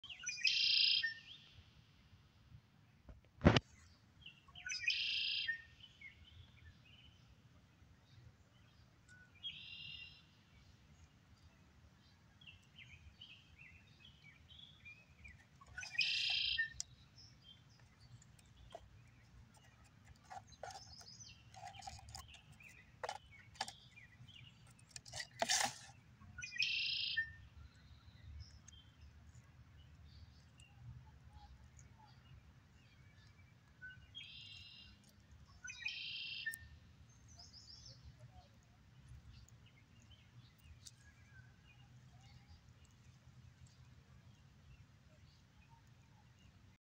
Canada Thistle Gall Fly
This is one of the coolest looking insects I've ever seen in person.